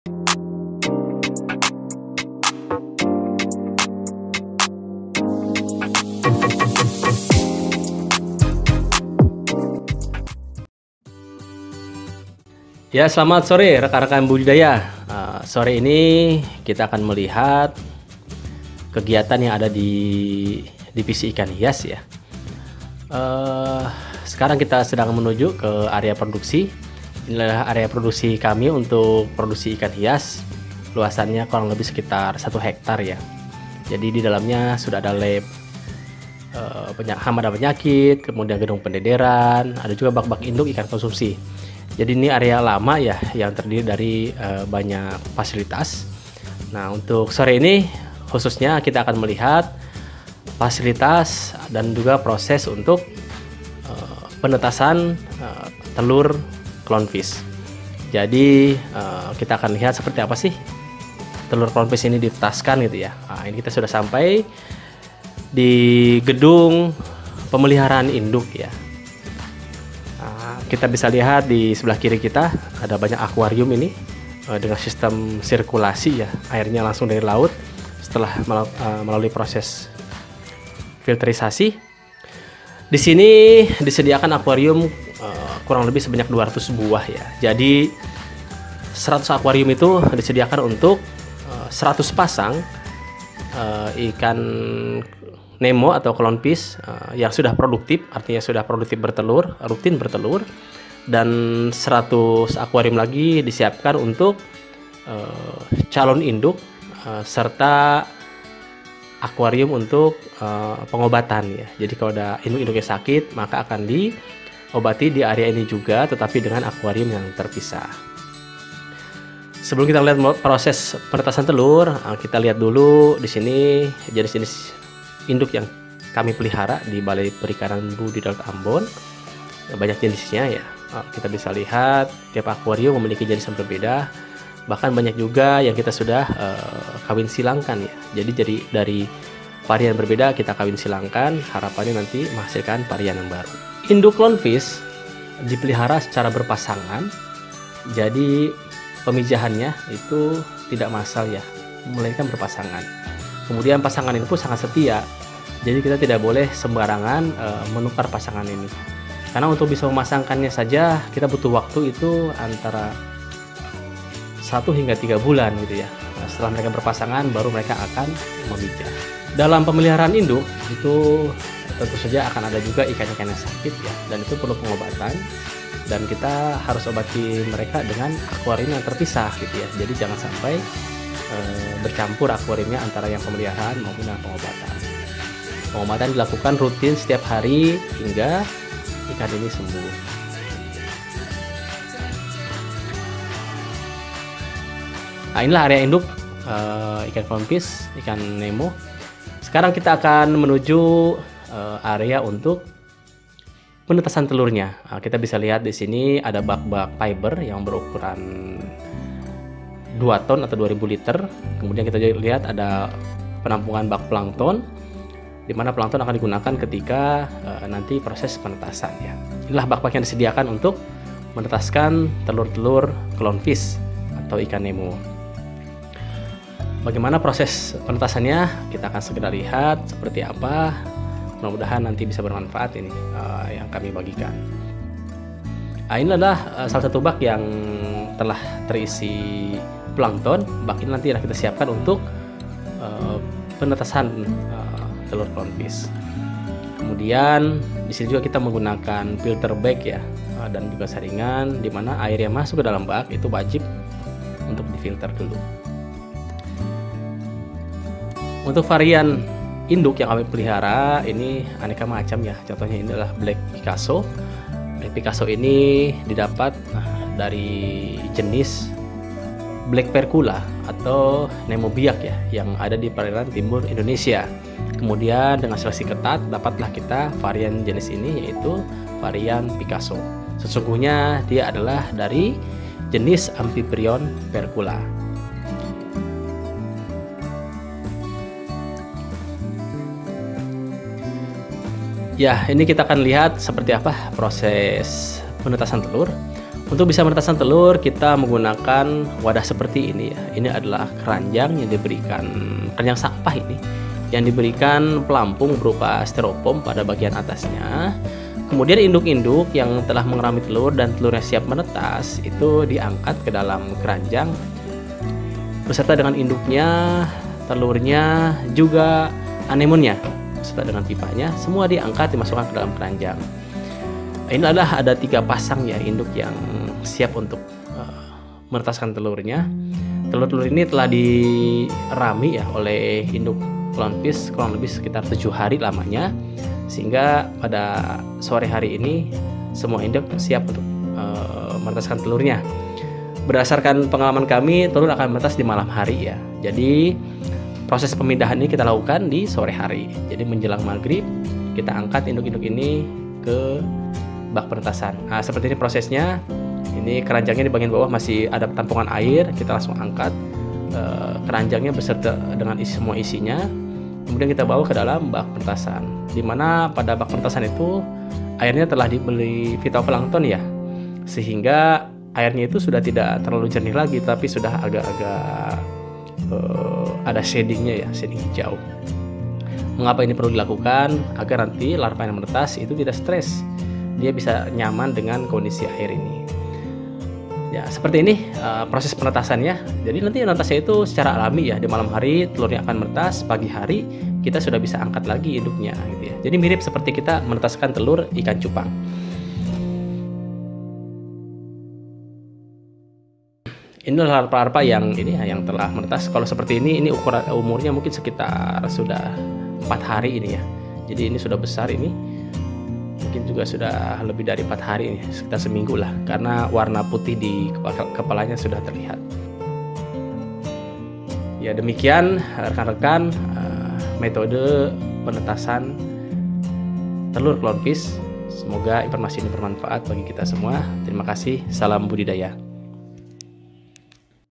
Metode Penetasan Telur Clownfish
Metode Penetasan Telur Clownfish di Balai Perikanan Budidaya Laut Ambon. video ini menggambarkan  cara untuk menetaskan telur-telur clownfish  secara baik dan benar. metode ini dipilih dan digunakan setelah melewati rangkaian aneka percobaan yang telah kami lakukan, dan kesimpulannya adalah metode penetasan inilah yang paling efektif dan efisien dalam pelaksanaan dan paling memuaskan untuk hasil yang didapat. semoga informasi ini bermanfaat bagi semuanya..